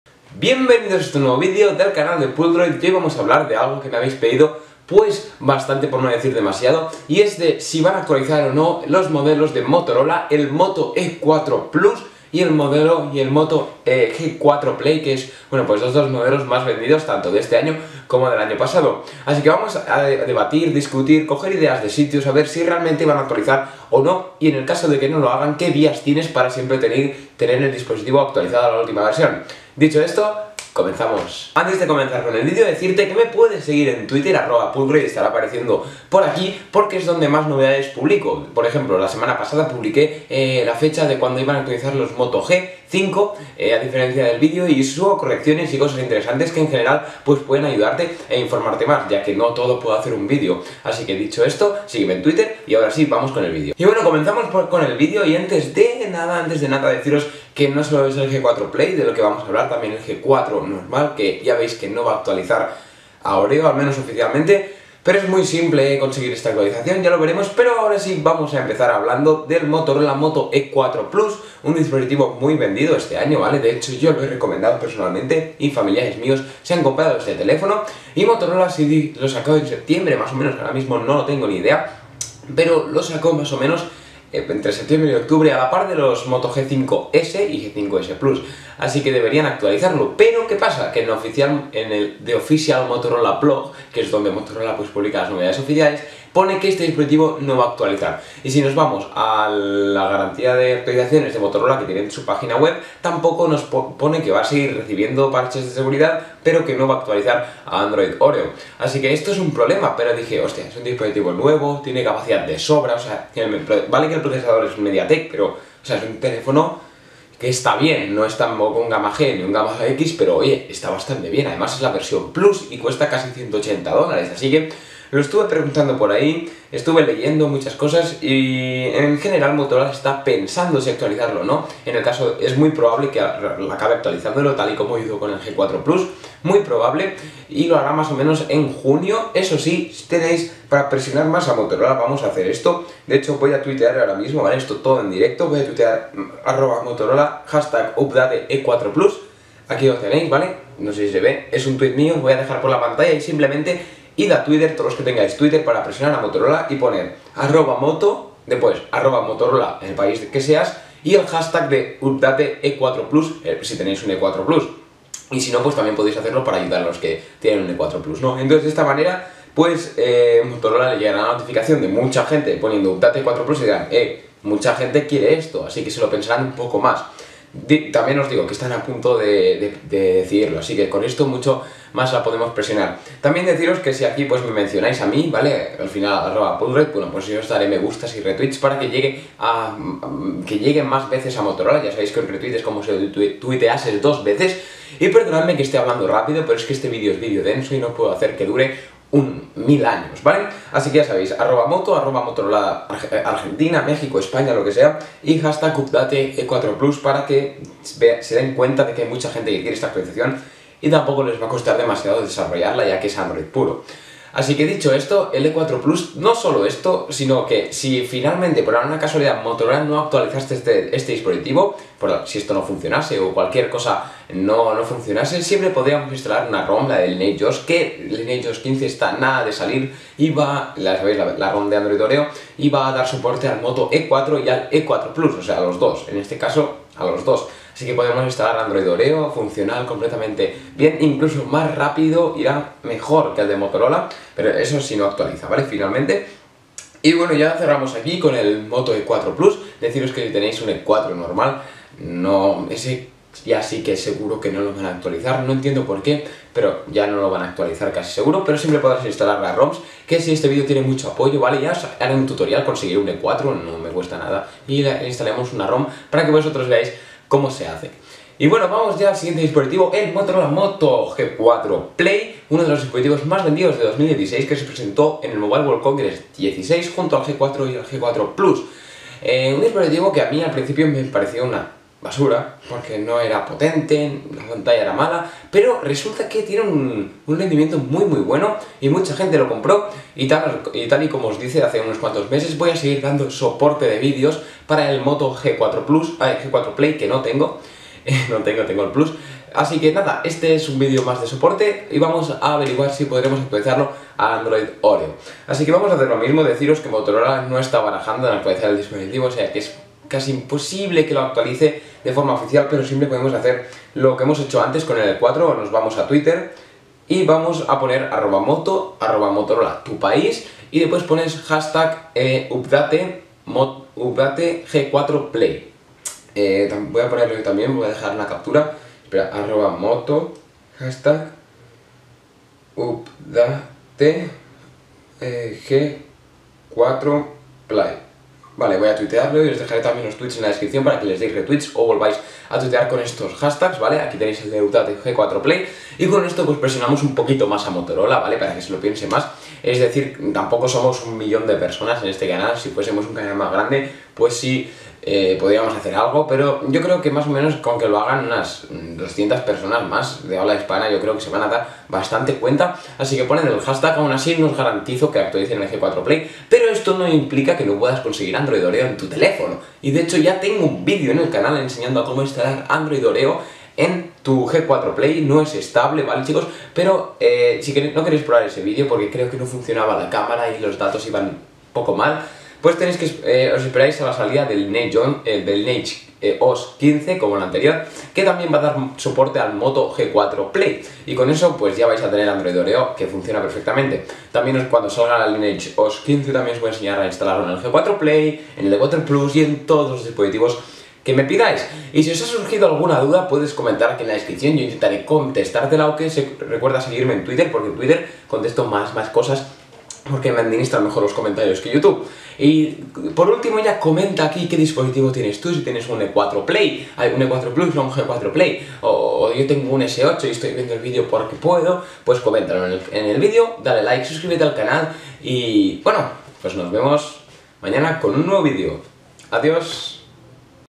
Actualización android oreo para moto e4 plus y moto g4 play (oficial)
Hoy os traigo un video informativo en el que hablo de la posible actualización a android 8.0 Oreo para los Motorola Moto E4 Plus y Moto G4/Play.